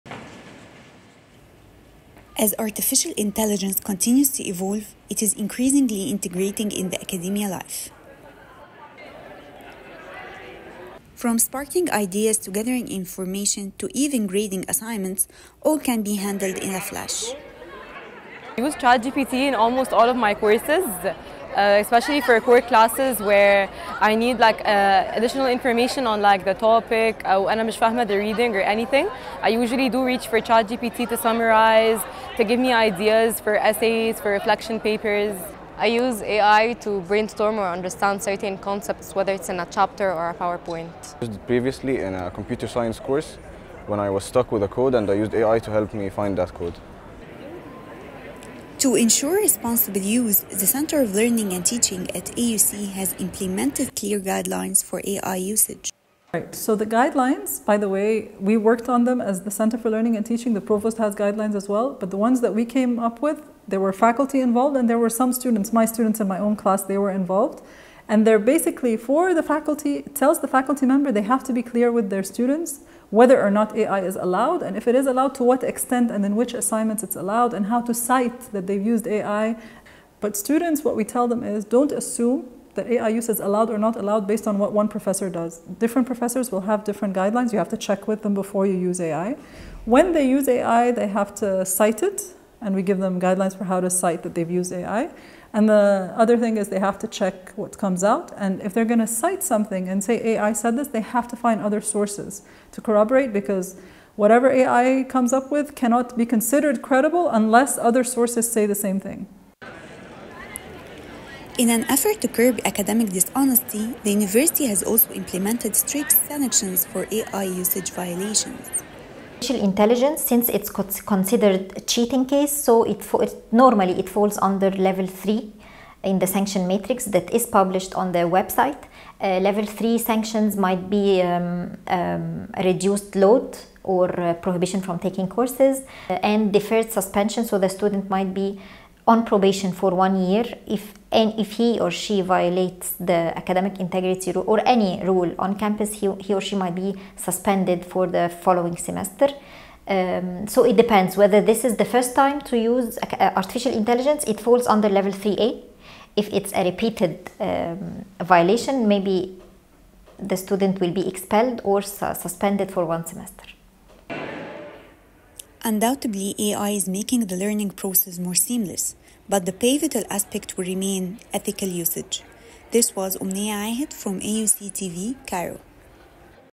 AI Guidelines and Sanactions
As artificial intelligence evolves, it is increasingly present in our lecture rooms and assignments. To ensure the proper use of it, AUC has uploaded clear guidelines on how AI can be used in a responsible way, respecting the boundaries of academic.